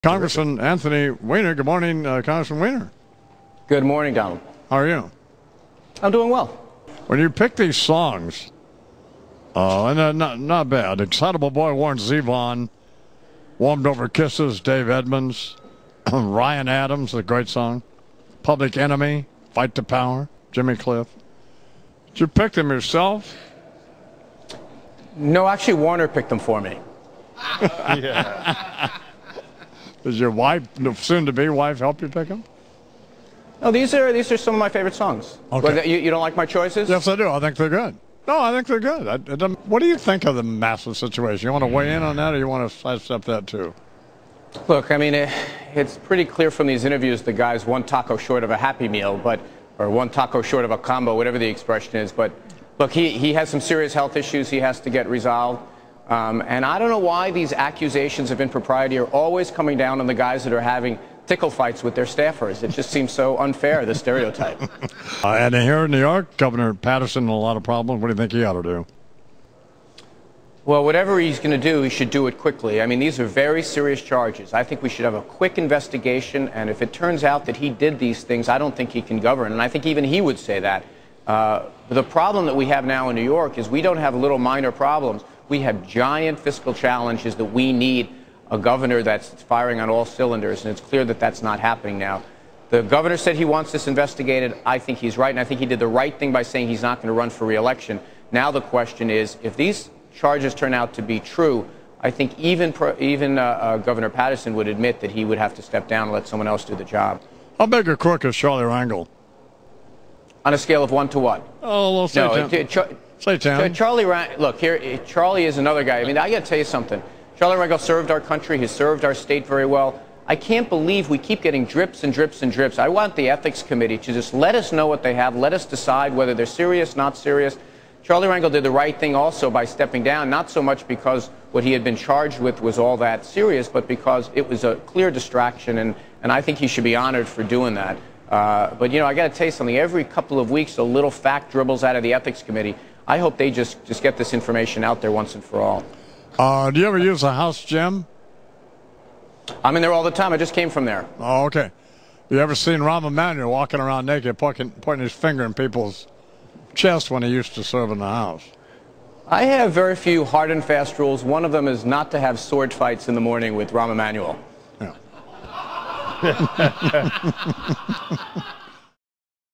Flashback: Weiner on David Paterson "He should step down"
From March 10, 2010. Anthony Weiner comments on Eric Massa, David Paterson, and Charlie Rangel scandals. Check out Weiner's favorite songs!